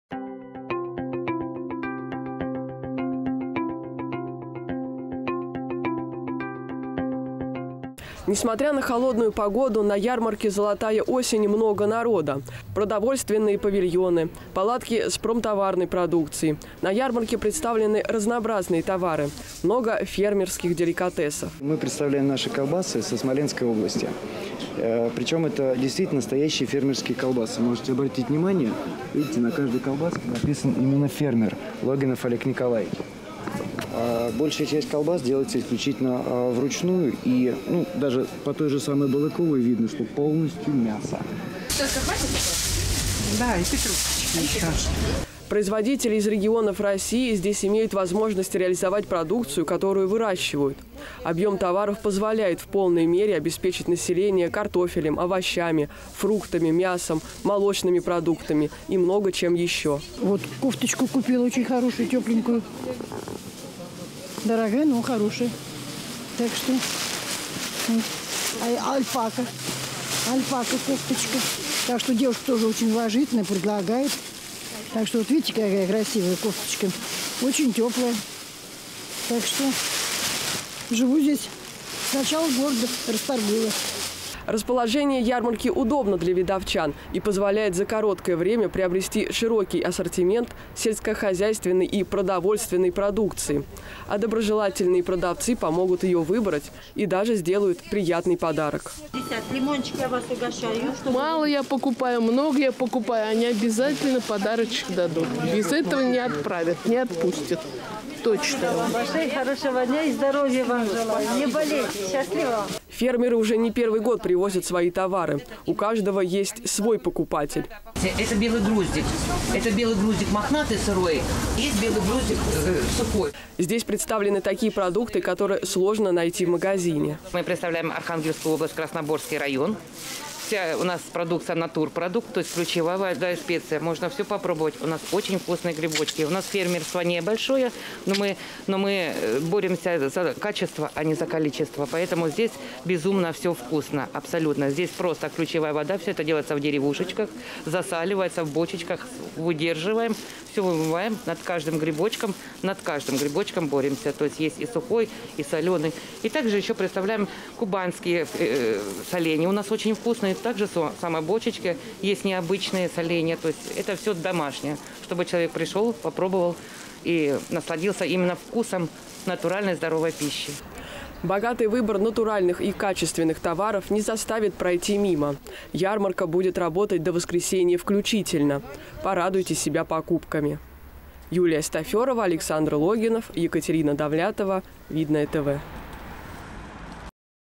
НА ЛЮБОЙ ВКУС
Около  кинотеатра «Искра» работает ярмарка «Золотая осень». Производители из разных регионов страны вновь привезли свою продукцию, чтобы видновчане могли приобрести качественные товары. Юлия Стафёрова присмотрелась к ценам.